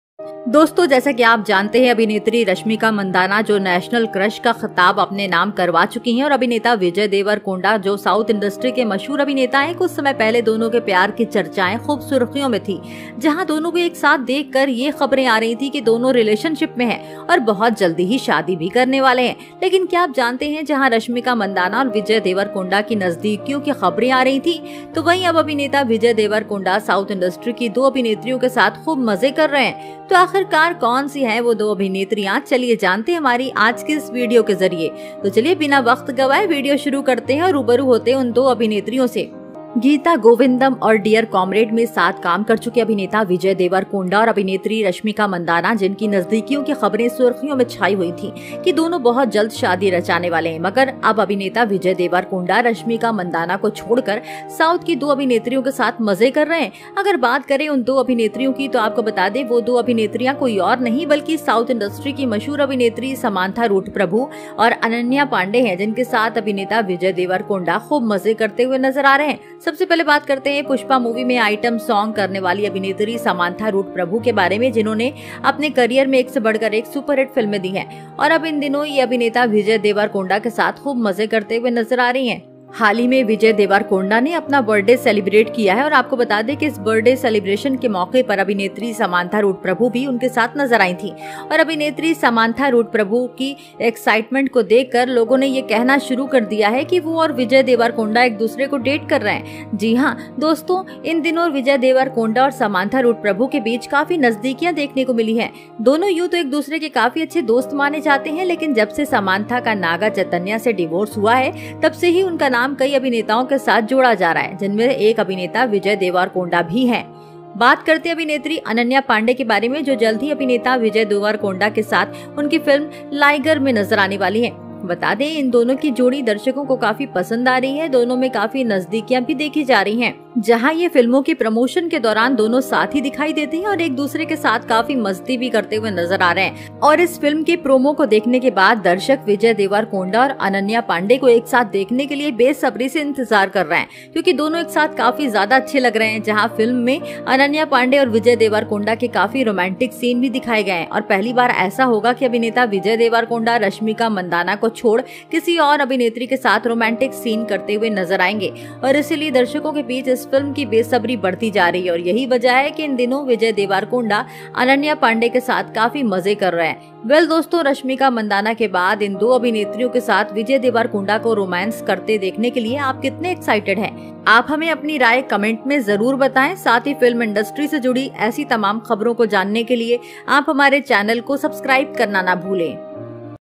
साउथ की मशहूर अभिनेत्री रस्मिका मंदाना को छोड़ विजय देवरकोंडा कर रहे इन दो अभिनेत्रियों के साथ मजे
except_Rashmika_Mandanna,_Vijay _Deverakonda_is_having_fun_with_these _two_actresses.

Friends, as you know, actress "Rashmika Mandanna" who has got the title of "National Crush", and actor "Vijay Deverakonda" who is a famous actor of South Industry, some time ago the discussions of their love were in the headlines. Where the news was coming after seeing both of them together, that bothIn a relationship and going to get married very soon.But do you know where the news of the closeness of "Rashmika Mandanna" and "Vijay Deverakonda" was coming, now the same actor "Vijay Deverakonda" is having a lot of fun with two actresses of South Industry, so who is he after all? Let's know two actresses, through our video, so let's go without wasting timeLet's start the video and get introduced to those two actresses.Actor "Vijay Deverakonda" and actress "Rashmika Mandanna", who worked together in "Geeta Govindam" and "Dear Comrade", whose closeness was in the news that both are going to get married very soon, but now actor "Vijay Deverakonda" Having fun with two actresses of South except "Rashmika Mandanna"If we talk about those two actresses, then let us tell you that those two actresses are none other than the famous actresses of South Industry "Samantha Ruth Prabhu" and "Ananya Pandey" with whom the actor "Vijay Deverakonda" is seen having a lot of fun. are.
First of all let's talk about the actress "Samantha Ruth Prabhu" who did "Item Song" in the movie "Pushpa", who has given more than one superhit film in her career, and now these days with the actor "Vijay Deverakonda". She is seen having a lot of fun, recently "Vijay Deverakonda" celebrated his birthdayCelebrated, and let us tell you that on the occasion of this birthday celebration, actress "Samantha Ruth Prabhu" was also seen with him, and seeing the excitement of actress "Samantha Ruth Prabhu", people started saying that, she is more " Vijay Deverakonda" are dating each other, yes, friends these days "VijayDevarakonda" and "Samantha Ruth Prabhu" have seen a lot of closeness, both are considered very good friends of each other, but ever since "Samantha Ruth Prabhu" got divorced from "Naga Chaitanya", since then His name is being associated with many actors, one of which is also actor Vijay DeverakondaIs.Let's talk about the actress "Ananya Pandey" who will soon be seen in her film "Liger" with actor "Vijay Deverakonda", let us tell you that the pair of these two is very much liked by the audience, and in both A lot of closeness is also being seen, where both of them together during the promotion of filmsThey are seen having a lot of fun with each other, and after watching the promo of this film, the audience is eagerly waiting to see "Vijay Deverakonda" and "Ananya Pandey" together. Because both of them are looking very good together, where in the film "Ananya Pandey" andA lot of romantic scenes of "Vijay Deverakonda" have also been shown and this will be the first time that actor "Vijay Deverakonda" will be seen doing a romantic scene with an actress other than "Rashmika Mandanna", and hence the audience is eagerly waiting for the film. is increasing, and this is the reason why these days Vijay Deverakonda is the "exclusivePandey".Well friends how excited are you to see Vijay Deverakonda romancing these two actresses after "Rashmika Mandanna"..? You must tell us your opinion by commenting in the comment box and do not forget to subscribe to our channel to know all such news related to the film industry